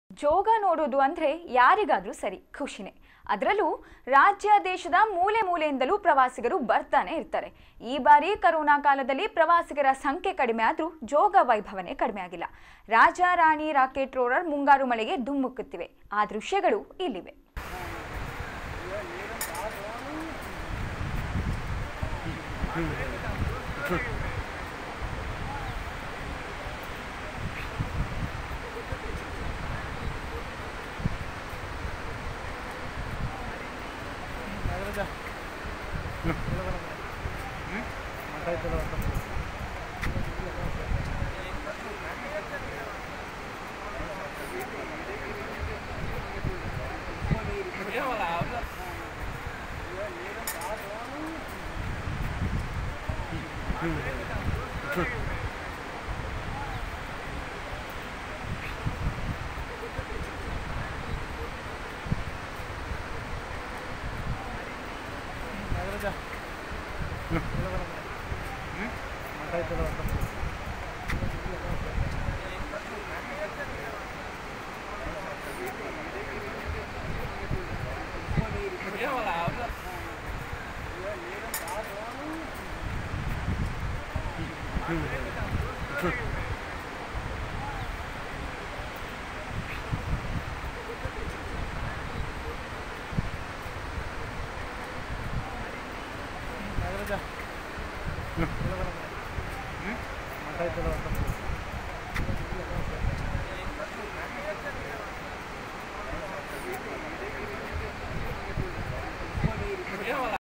ಮರುಕಳಿಸಿದ ಜೋಗ ವೈಭವ
ಜೋಗ ನೋಡೋದು ಅಂದ್ರೆ ಯಾರಿಗಾದ್ರೂ ಸರಿ ಖುಷಿನೇ.. ಅದ್ರಲ್ಲೂ ರಾಜ್ಯ, ದೇಶದ ಮೂಲೆ ಮೂಲೆಯಿಂದಲೂ ಪ್ರವಾಸಿಗರು ಬರ್ತಾನೆ ಇರುತ್ತಾರೆ. ಈ ಬಾರಿ ಕರೋನಾ ಕಾಲದಲ್ಲಿ ಪ್ರವಾಸಿಗರ ಸಂಖ್ಯೆ ಕಡಿಮೆಯಾದ್ರೂ ಜೋಗ ವೈಭವವನೇ ಕಡಿಮೆಯಾಗಿಲ್ಲ. ರಾಜ, ರಾಣಿ, ರಾಕೆಟ್, ರೋರರ್ ಮುಂಗಾರು ಮಳೆಗೆ ಧುಮ್ಮಿಕ್ಕುತ್ತಿವೆ. ಆ ದೃಶ್ಯಗಳು ಇಲ್ಲಿವೆ.